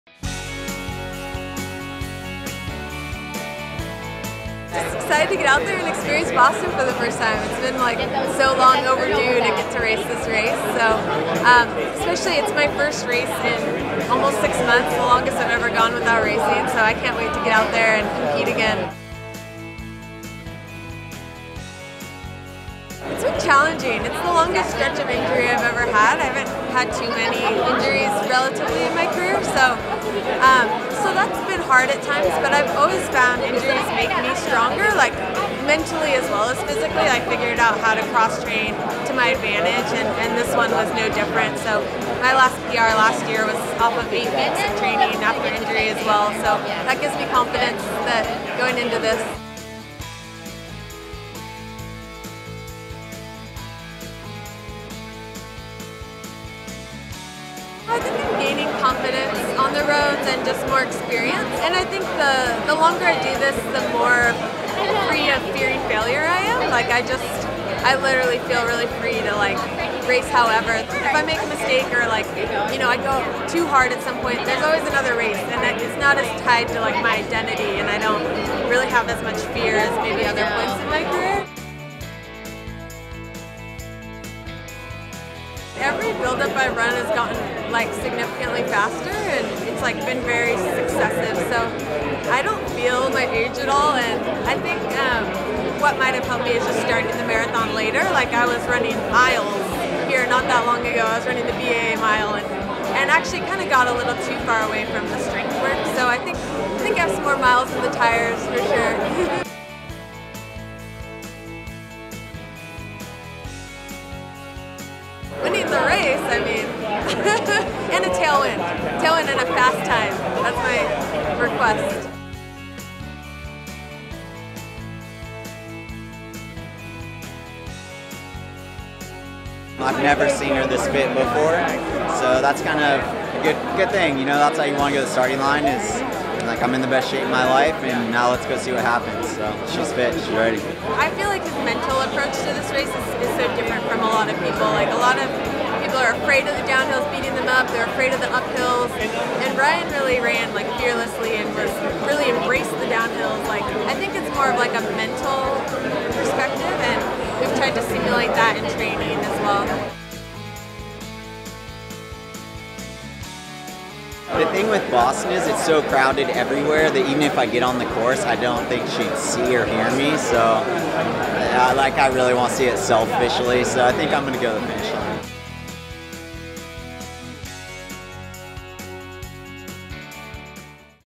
2019 Boston Marathon Preview: Sara Hall
After setting new PRs in 2018, Sara Hall is ready for her Boston Marathon debut.
xxxx
SARA
Just excited to get out there and experience Boston for the first time. It’s been like so long overdue to get to race this race. It’s my first race in almost six months, The longest I’ve ever gone without racing, so I can’t wait to get out there and compete again.
Hall was nursing an injury in her peroneal tendon.
SARA
It’s been challenging. It’s the longest stretch of injury I’ve ever had, I haven’t had too many injuries relatively in my career. So that’s been hard at times. But I’ve always found injuries make me stronger, mentally as well as physically. I’ve figured out how to cross-train to my advantage and this one was no different. My last PR last year was off of eight weeks of training after injury as well, so that gives me confidence going into this.
She set a new PR of 2:26:20 in Ottawa last May.
Hall also set a half marathon PR of 1:09:27.
SARA
You know, I think I’m gaining confidence on the roads, and more experience. And I think the longer I do this, the more free of fearing failure I am. I literally feel really free to race however. If I make a mistake or I go too hard at some point, there’s always another race. It’s not as tied to my identity and I don’t have really as much fear as maybe other points in my career.
The 14-year vet has no plans of stopping.
SARA
Every buildup I run has gotten significantly faster and it’s been very successive. I don’t feel my age at all and I think what might have helped me is just starting the marathon later. Like I was running miles here not that long ago, I was running the BAA Mile and actually got a little too far away from the strength work. I think I have some more miles left in the tires for sure.
Hall turns 36 on race day.
Her perfect birthday present?
SARA
Winning the race. And a tailwind. Tailwind and a fast time, that’s my request.
Hall is coached by her husband Ryan.
Ryan Hall ran a 2:04:58 on the course in 2011.
RYAN
I’ve never seen her this fit before. So that’s a kind of a good thing. That’s how you want to get to the starting line, like I’m in the best shape of my life, now let’s go see what happens. She’s fit, she’s ready.
SARA
I feel like his mental approach to this race is so different from a lot of people. A lot of people are afraid of the downhills beating them up, they’re afraid of the upnhills, and Ryan really ran fearlessly and really embraced the downhills. I think it’s more of a mental perspective and we’ve tried to simulate that in training as well.
On race day, he’ll be more husband than coach.
RYAN
The thing with Boston is it’s so crowded everywhere that even if I could get on the course, I don’t think she’d see or hear me. I really want to see it selfishly, so I think I’m going to go to the finish line.